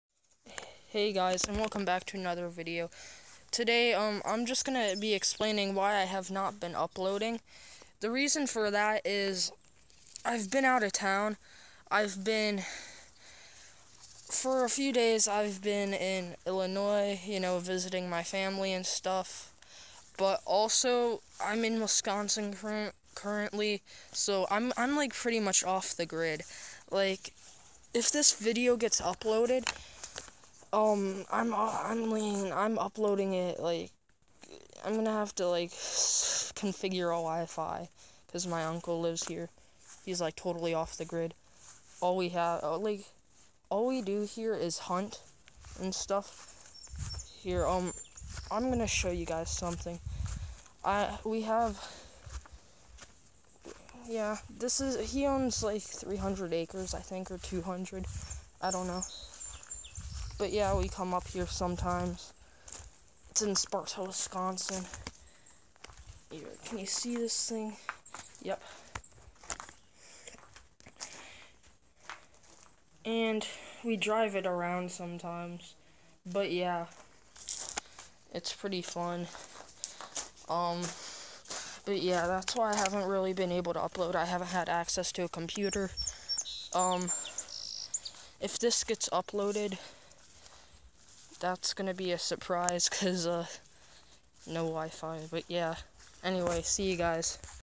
Why I have not been uploading
Well I am using the worst wifi in the world to upload this. More videos planned.